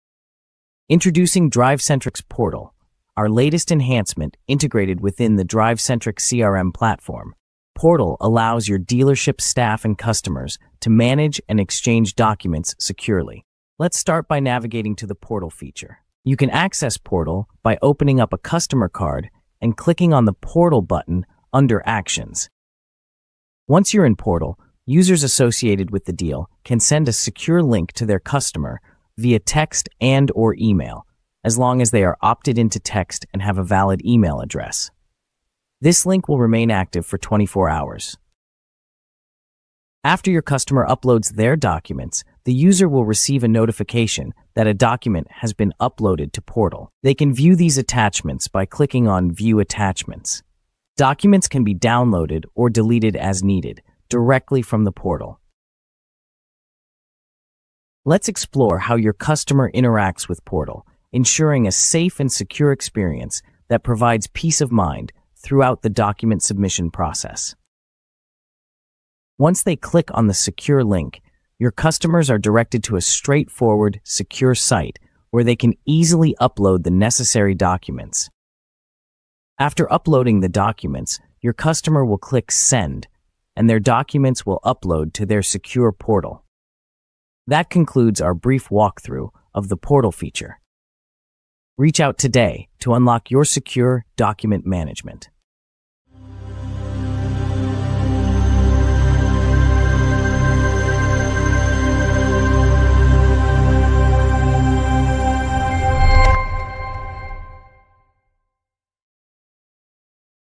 DriveCentric CRM Portal: Car Dealership Document Management Made Easy
In this video, we introduce DriveCentric Portal, the latest enhancement integrated within the DriveCentric CRM platform. The Portal feature enables both dealership staff and customers to securely manage and exchange important documents. We’ll guide you through how to access Portal from a customer card and send a secure link via text or email for easy document submission.

Learn how dealership staff can receive notifications when documents are uploaded, view attachments, and manage them directly from the portal. We’ll also demonstrate the customer’s experience, showcasing how they can securely upload documents with ease. This streamlined, secure process provides peace of mind for both staff and customers during document exchanges. Discover how DriveCentric Portal simplifies document management and enhances your dealership’s workflow!